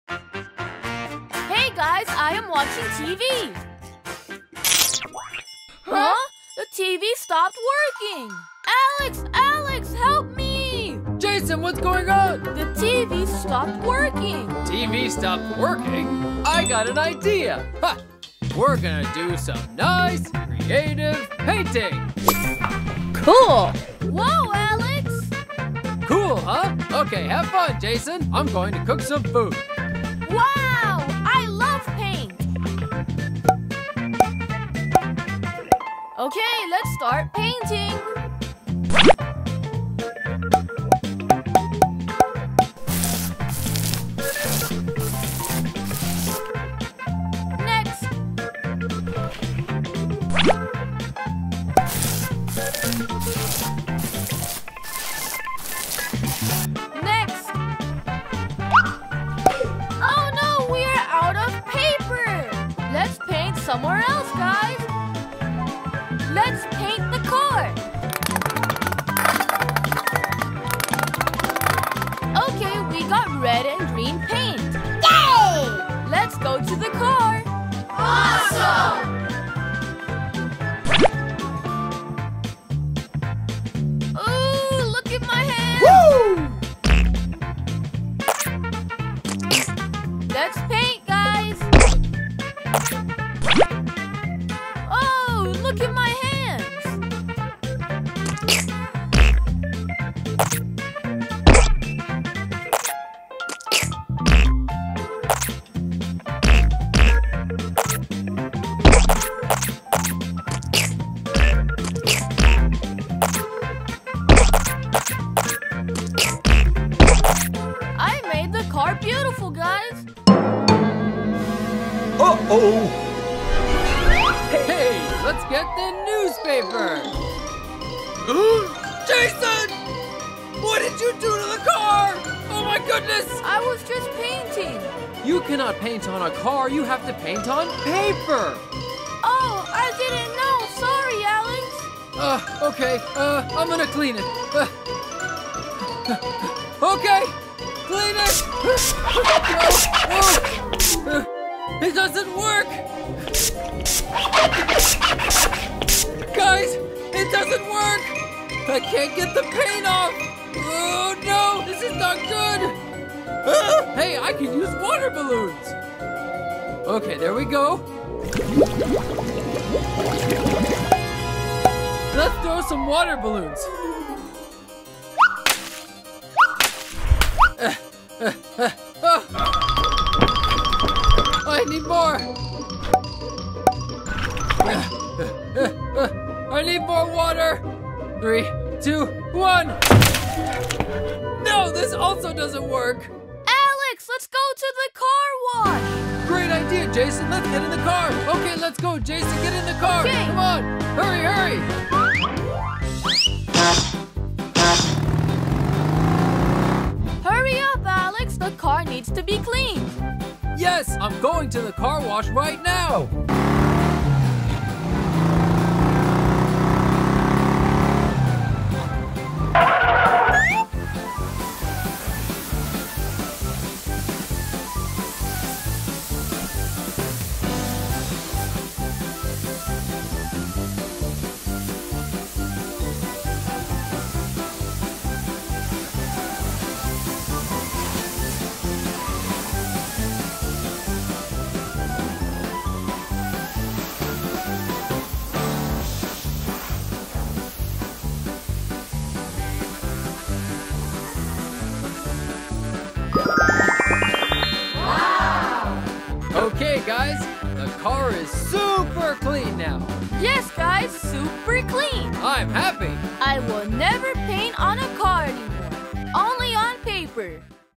Jason cleans the car at real carwash with Alex
Jason goes to a real carwash with his brother